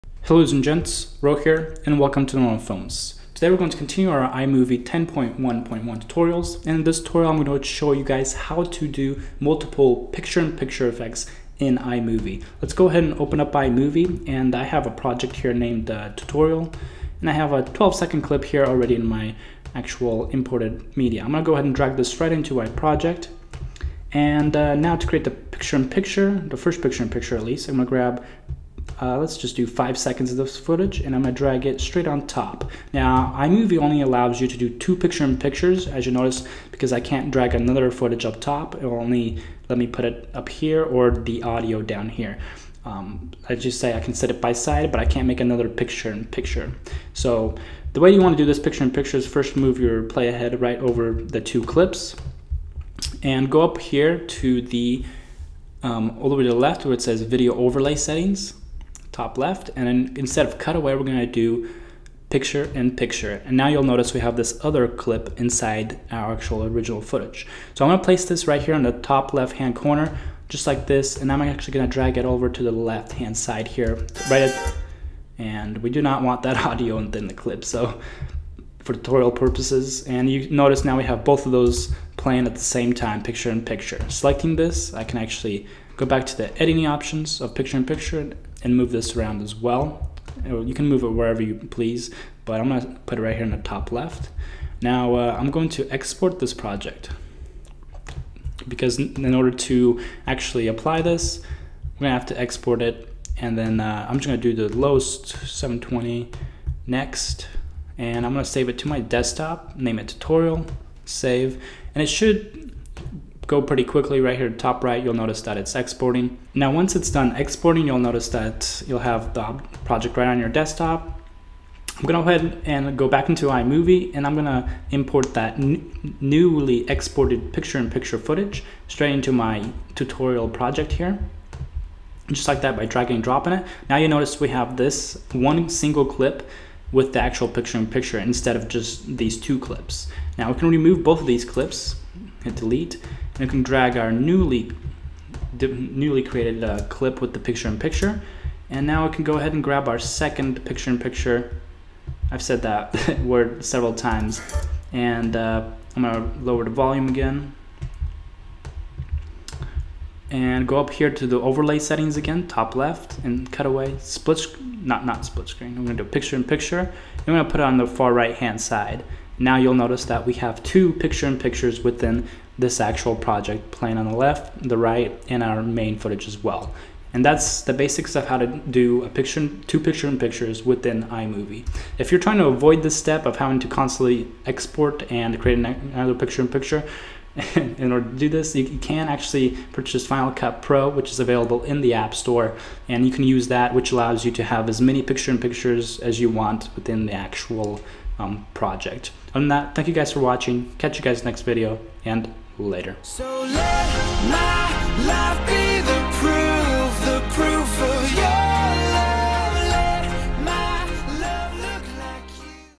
Multiple Picture in Pictures in iMovie 10.1.1 | Tutorial 81
This video shows you how to have more than one Picture in Picture effects through iMovie.  Allowing you to have multiple Picture in Pictures in your final video.
Edit - Overlay Settings - Picture in Picture